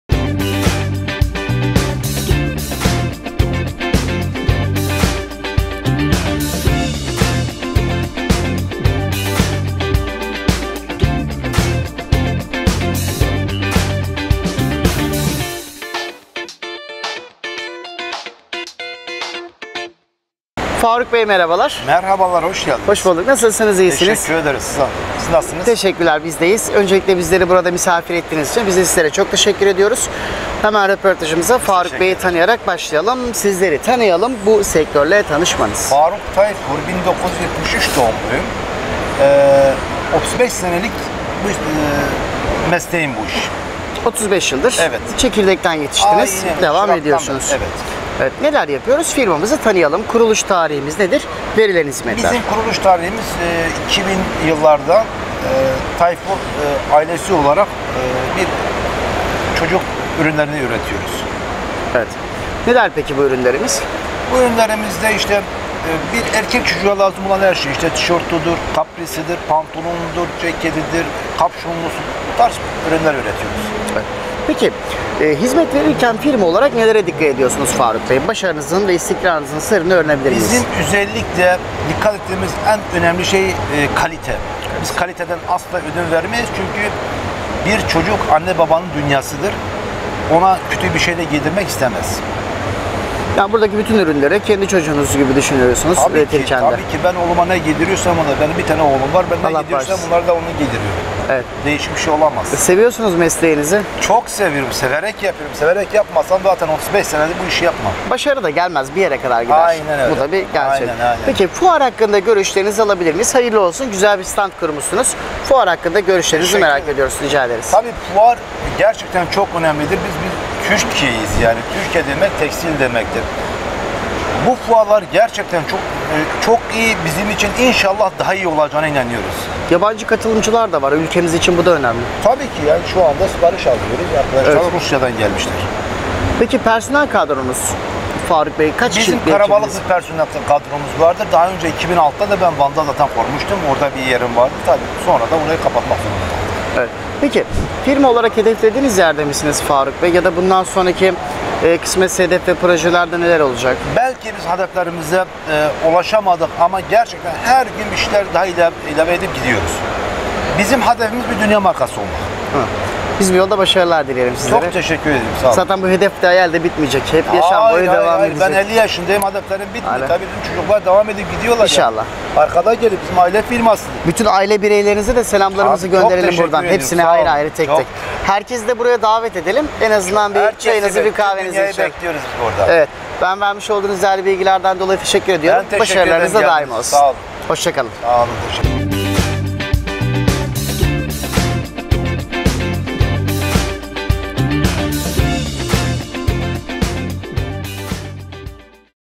DOFFBI - CNR CBME TURKEY - ANNE BEBEK FUARI
İstanbul ili Fatih İlçesinde faaliyette bulunan DOFFBI Firma Sahibi Sn. Faruk TAYFUR, başarı ve faaliyetlerini CNR CBME TURKEY - ANNE BEBEK FUARI firma standında, Fuar TV muhabirimiz Sinan TOPÇUOĞLU' e anlattı.